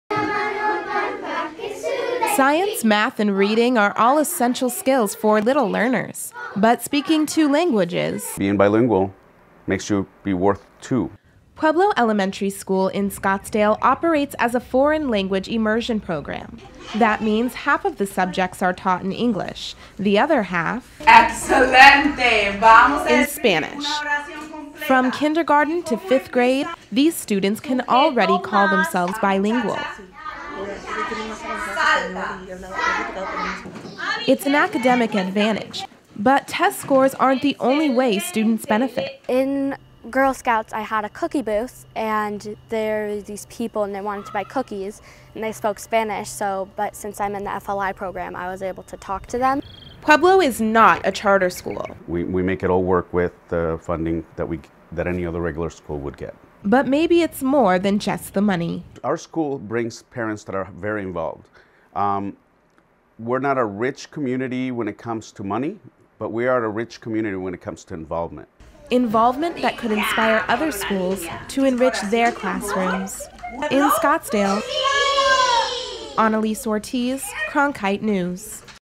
Pueblo Elementary School students are immersing themselves in a second language
Language immersion might sound like moving to a foreign country as an adult, but the language immersion program at Pueblo Elementary School is something quite different. Cronkite News reporter Analise Ortiz has more on this Scottsdale school's program.